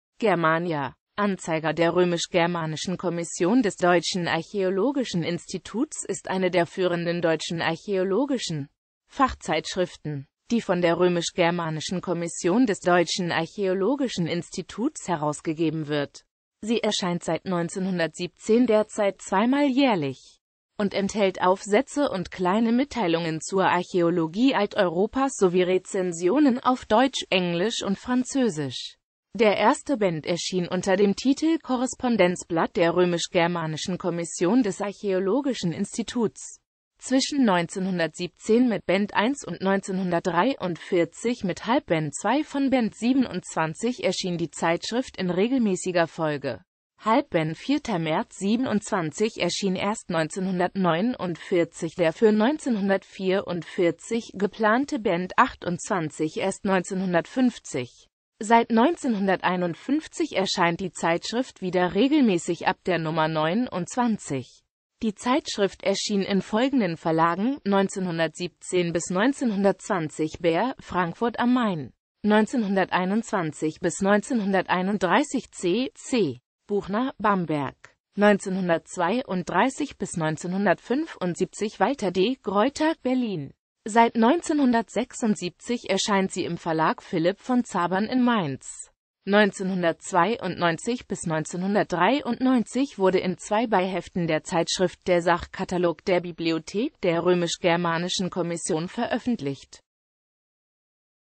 Germania (Zeitschrift)
Vielen Dank für Ihre Unterstützung:

Germania.Anzeiger der Römisch-Germanischen Kommission des Deutschen Archäologischen Instituts ist eine der führenden deutschen archäologischen Fachzeitschriften, die von der Römisch-Germanischen Kommission des Deutschen Archäologischen Instituts herausgegeben wird.Sie erscheint seit 1917, derzeit zweimal jährlich, und enthält Aufsätze und Kleine Mitteilungen zur Archäologie Alteuropas sowie Rezensionen auf Deutsch, Englisch und Französisch.Der erste Band erschien unter dem Titel Korrespondenzblatt der Römisch-Germanischen Kommission des Archaeologischen Instituts.